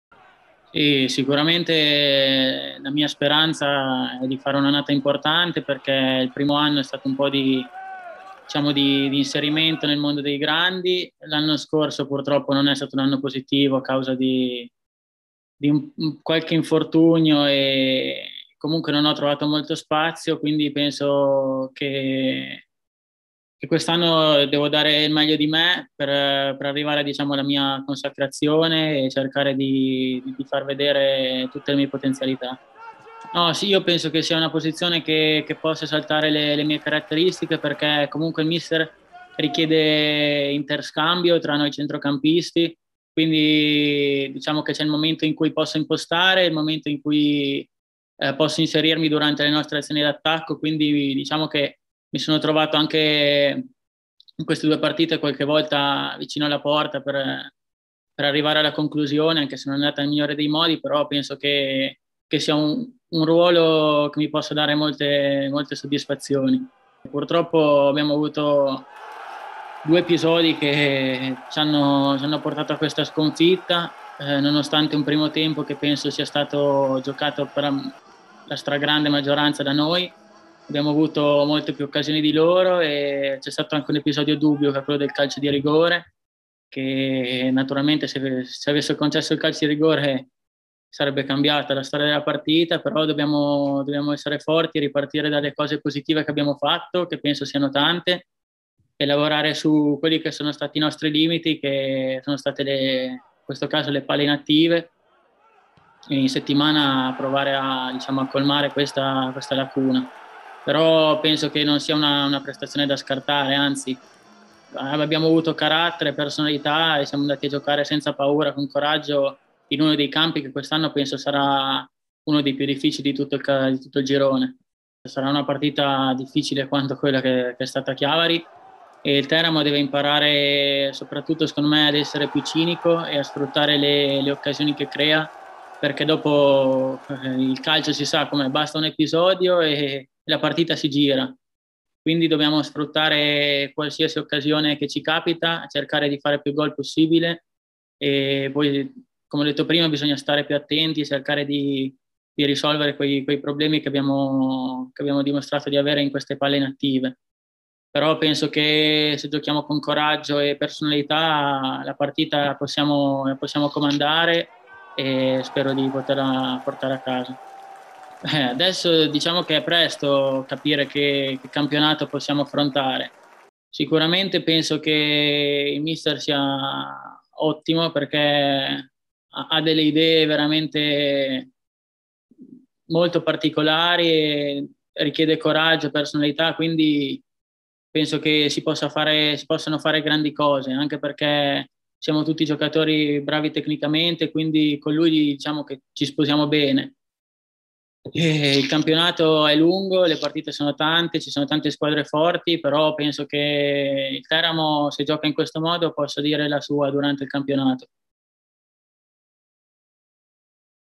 Teramo. Viero alla prova della maturità
Alla terza stagione in maglia biancorossa, per Federico Viero è arrivato il momento di prendersi lo spazio nella mediana di Guidi. Per il centrocampista parmense prima da titolare a Chiavari e lo sguardo adesso va all’impegno di domenica pomeriggio contro il Siena